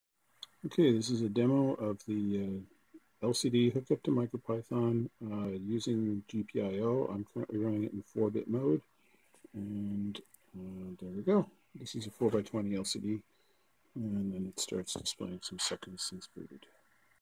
MicroPython GPIO LCD
This is a demo of MicroPython driving an HD44780 style 4 line by 20 column LCD using GPIO. This particular demo was done using 4-bit mode.